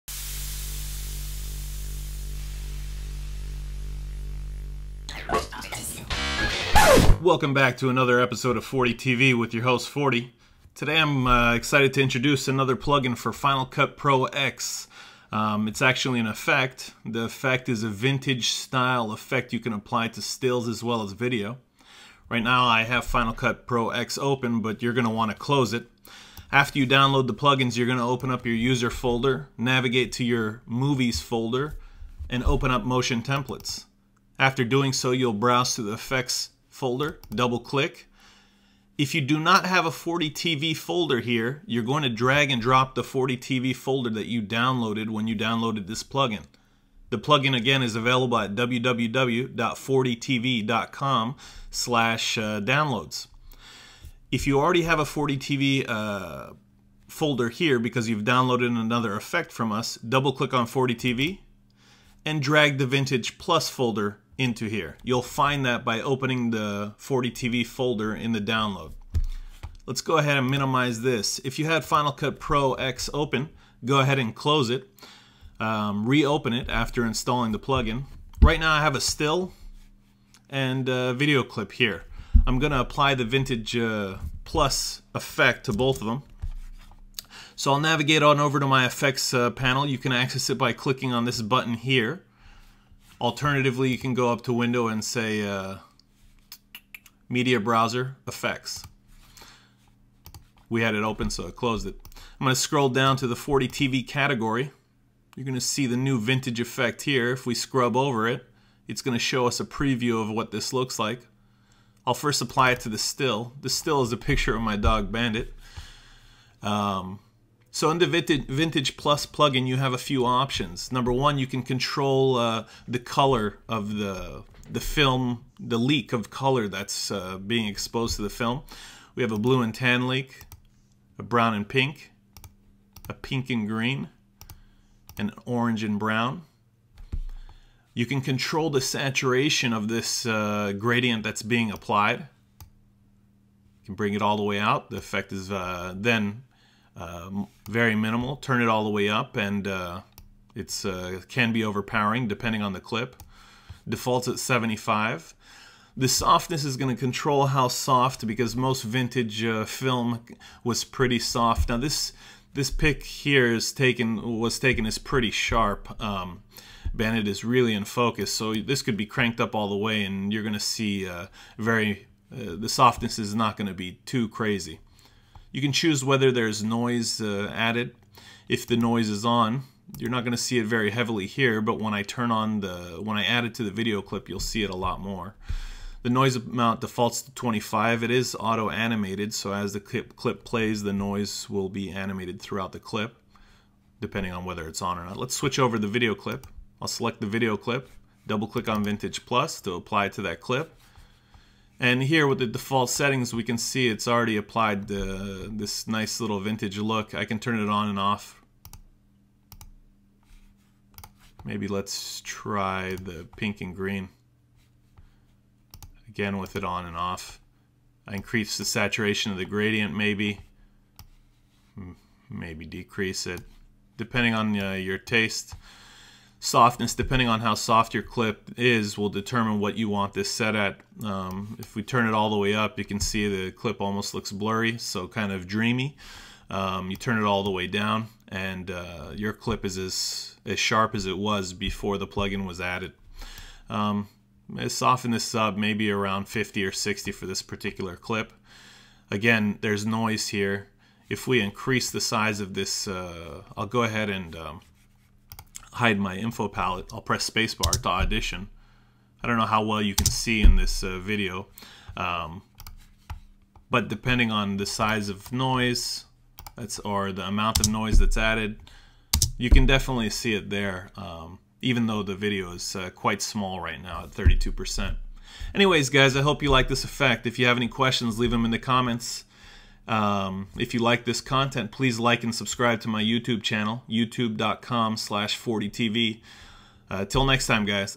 FREE FCPX Effect - Vintage Plus Effect for Final Cut Pro X (FCPX)
Video Tutorials For Beginners-Advanced Using Final Cut Pro X (FCPX)

This video shows you how to use FordeeTV's Vintage Plus Effect for Final Cut Pro X (FCPX). You can download this effect for free from our

You can set different color film leaks and their saturation, control softness and determine whether you have added noise and how much.